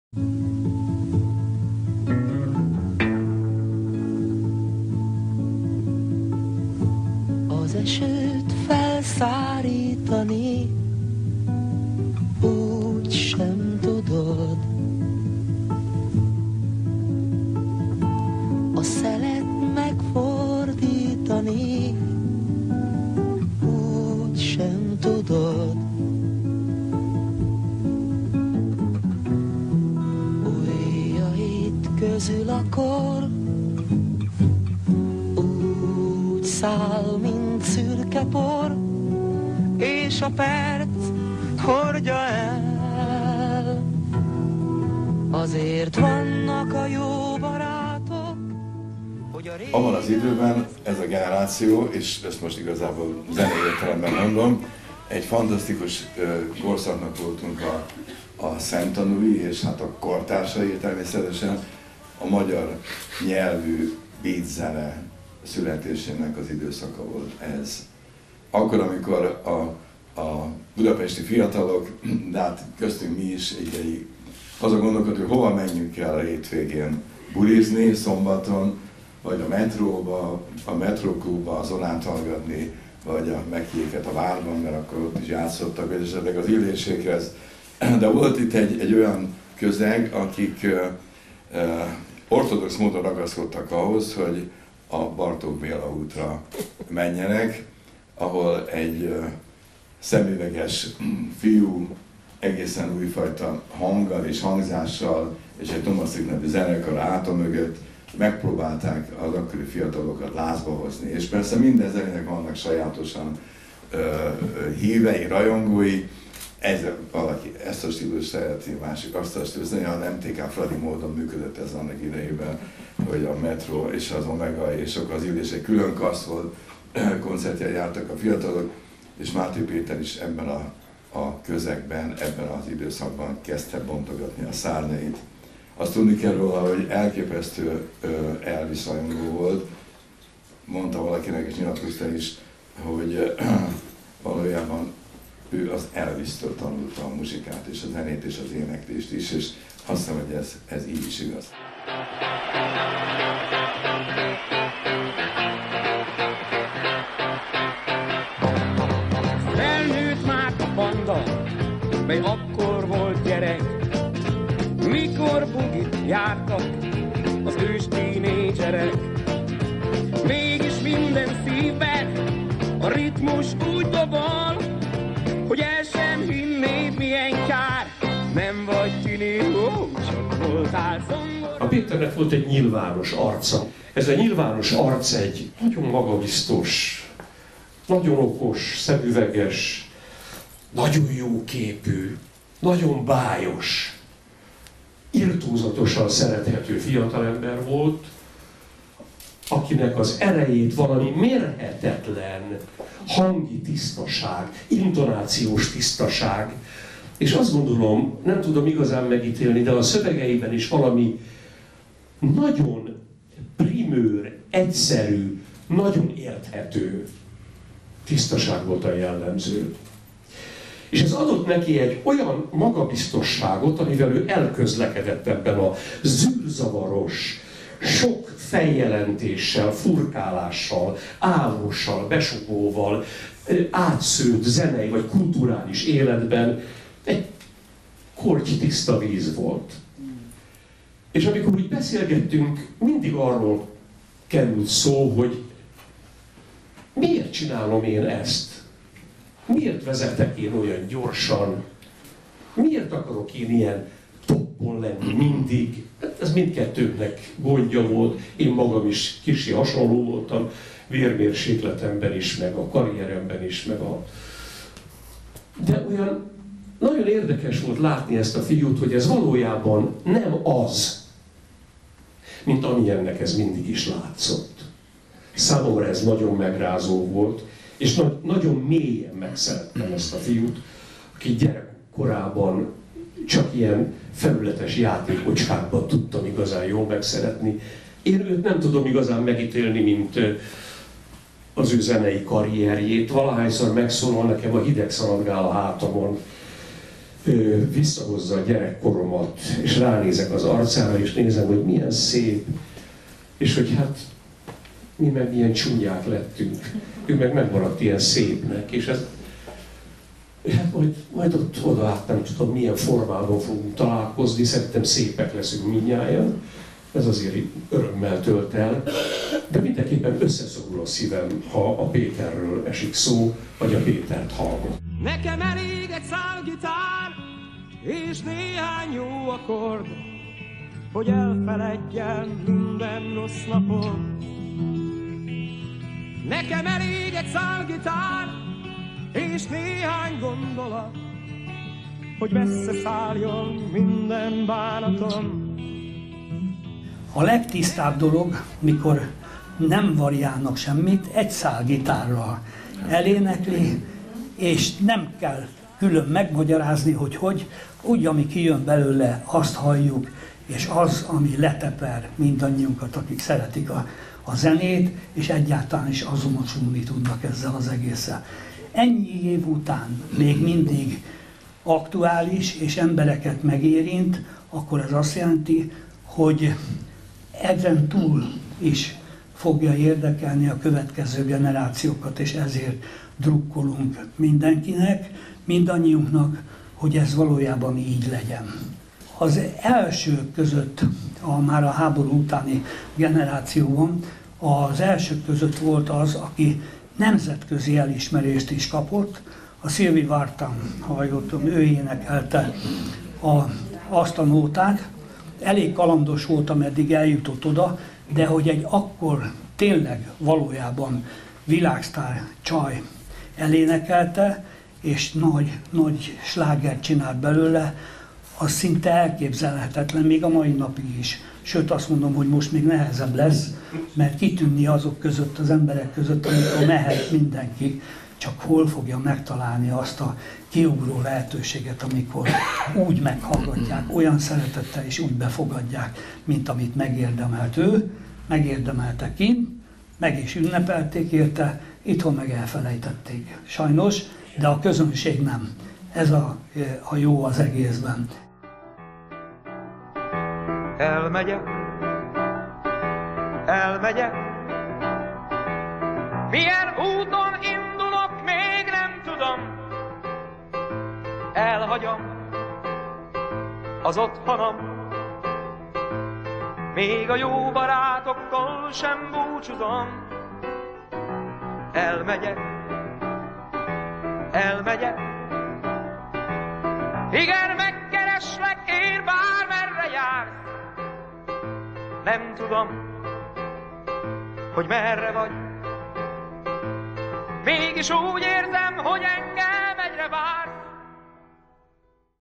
Tér-Kép Galéria - Máté Péter emlékest
Az esten Benkő László, Kossuth-és Liszt Ferenc-díjas zenész, az Omega együttes billentyűse mellett Márton András, Jászai Mari-díjas színművész, rendező és Soltész Rezső, énekes, zeneszerző idézték fel emlékeiket Máté Péterről.